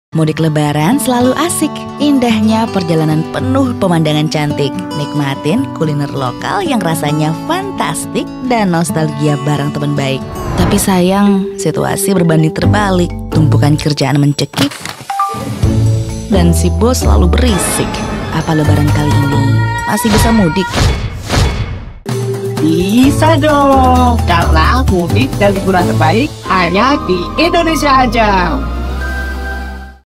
TerpesonaRamadan Edisi Mudik 2024
Perjalanan yang seru, lingkungan yang asik, dan nikmatin kebudayaan yang kental emang sesuatu yang bikin rindu sama kampung halaman yaa, Sobat Pesona.

Walaupun pekerjaan lagi sibuk-sibuknya, yuk kita luangkan waktu untuk bertemu keluarga dan menyambut hari kemenangan bersama-sama. Yuk, mudik dan buat momen liburan